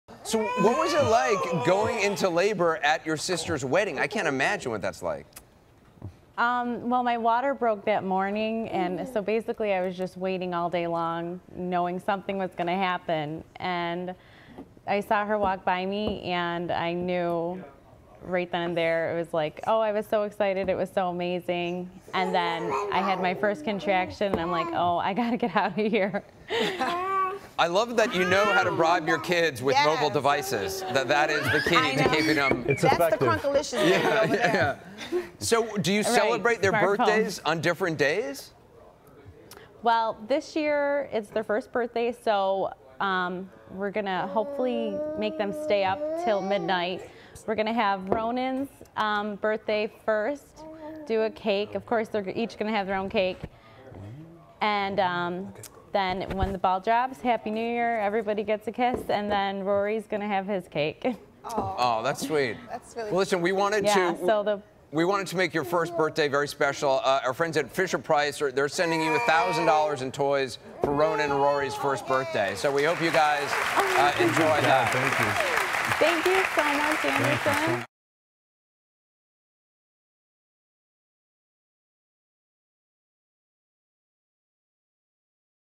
Rosputni Twins Born in Different Years
The Rosputni family appeared on today's show to talk about their twins Rory and Ronan, born in different years.